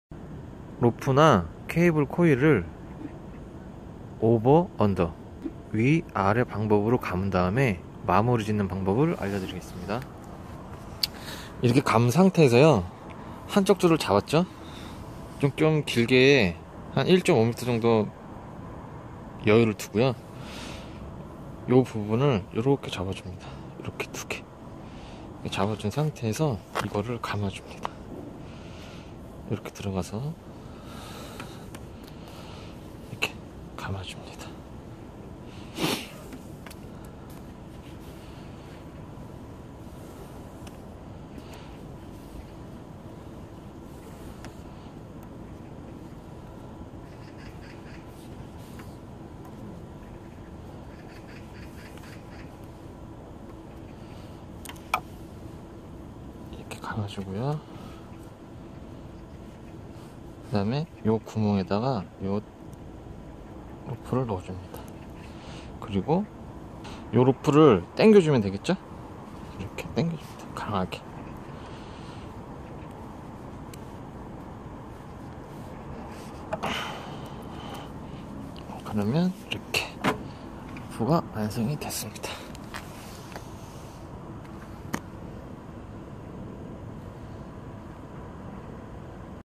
로프종류및산업현장케이블코일 꼬이지않게 감기후 마무리 보관하는방법
로프 꼬이지않게 감기후 보관하는방법입니다.